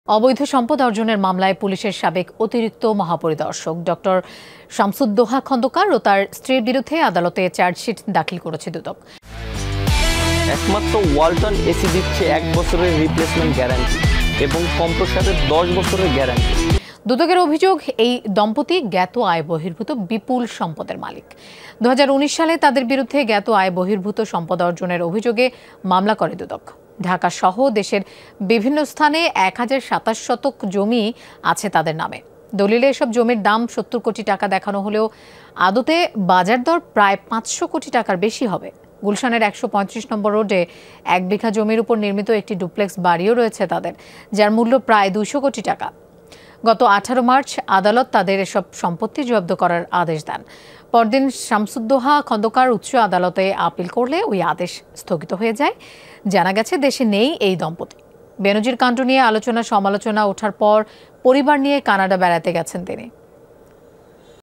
অবৈধ সম্পদের মামলায় সাবেক এআইজি শামসুদ্দোহার বিরুদ্ধে চার্জশিট | DUDOK | AIG Corruption | Jamuna TV
অবৈধ সম্পদ অর্জনের মামলায় পুলিশের সাবেক অতিরিক্ত মহাপরিদর্শক ড. শামসুদ্দোহা খন্দকার ও তার স্ত্রীর বিরুদ্ধে আদালতে চার্জশিট দাখিল করেছে দুদক। দুদকের অভিযোগ, এ দম্পতি জ্ঞাত আয়বহির্ভুত বিপুল সম্পদের মালিক।

Address:
Bangladesh.